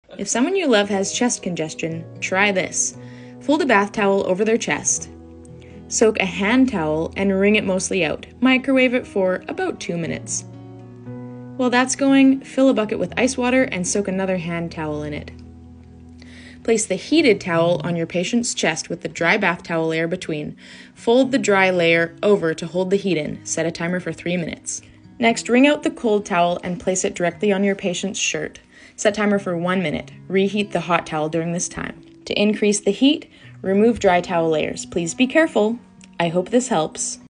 Hydrotherapy for Immune Health | Hot and Cold Contrast for Chest Congestion
Benefits: Increases circulation to the chest and helps loosen mucous in the lungs, making it easier to eliminate.
Consult Your Doctor First If: You have uncontrolled high blood pressure or heart conditions such as Congestive Heart Failure, or are at risk for stroke.

Caution: If pregnant, monitor overall body temperature and modify treatment if needed. Equipment: 1 bath towel, two hand towels, a loose t shirt, a helper, a timer, a basin and a microwave

This treatment is great for soothing a dry cough or helping you clear your lungs during an upper respiratory illness. My mother would do this on us when we were sick, and I have administered this treatment to friends and family alike.

You will be the patient for this one. Your helper will be doing the work. It’s time to get comfy!

The Patient’s job: Remove undergarments and put on a loose-fitting t-shirt (this is to create a barrier, not for privacy). Lie down on your back on the couch or bed. Place the bath towel over your chest, folded in half to cover your chest twice.

The Helper’s job: Prepare a hot compress by wetting one of the hand towels and microwaving it for 1-3 min (Usually 2.5 minutes, depending on your microwave). Ensure that you wring the towel most of the way out before microwaving it, and that you check the temperature at the one and two minute marks. When the towel is nice and hot (but you can still handle it with your bare hands briefly), it’s ready to go!

In the meantime, fill your basin with cold water and ice. Soak the second hand towel in the ice water.

Remove the hot towel from the microwave. Place it on the chest of the patient with the dry bath towel between, acting as a barrier. I like to also cover the compress with the excess of the dry towel to keep the heat in. If the patient does not feel the heat in ~30 seconds, unfold the dry bath towel so that there is only one dry towel layer between the heat and the patient. If that is still not hot enough, remove the dry towel entirely and place the hot hand towel directly over the t-shirt (this can usually be done by about round 2 or 3 as the patient gets used to the heat.). Leave the hot towel on for 3 minutes.

Wring out the cold towel most of the way and place it on the patient’s chest directly on the t-shirt layer. Leave for 1 minute.

Reheat the hot towel in the microwave for 1 minute during the cold application, redampening as needed.

Remove the cold towel and replace it with the reheated hot towel. Return the cold towel to the ice water during the heat application.

Repeat 4-6 times, alternating the hot for 3 minutes and the cold for 1 minute, and reheating and rechilling the opposite towel during applications. End with a cold application, and then have the patient dry off and put on a dry shirt.

This can be repeated a couple times a day as needed, so long as you allow 2-3 hours between applications.